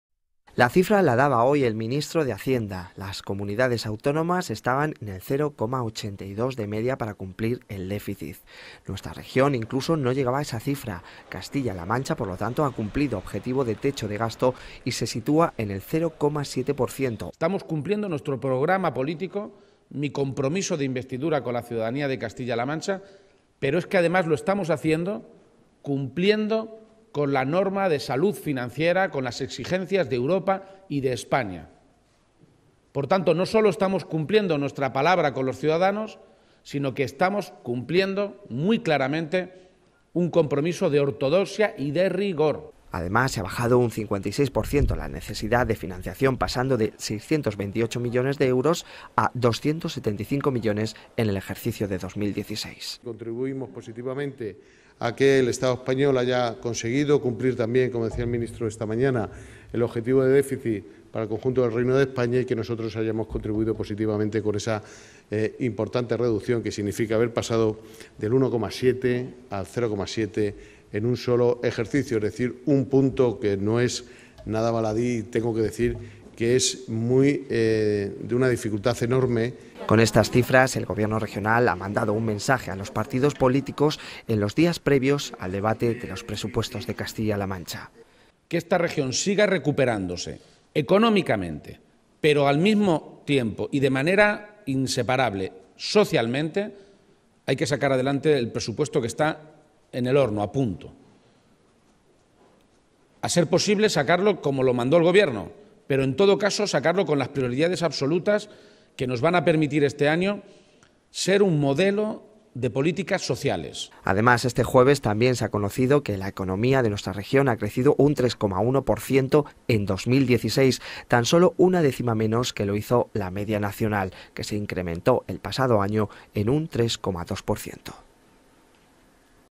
CASTILLA-LA MANCHA CUMPLE CON EL OBJETIVO DE DÉFICIT
Desciende el déficit en nuestra región del 1.7 de 2015 a un 0.7 del pasado año.  Castilla-La Mancha esta por debajo de la media nacional, ya que las comunidades autónomas sitúan su déficit en torno al  0,82. Son datos presentados hoy por el Ministerio de Hacienda .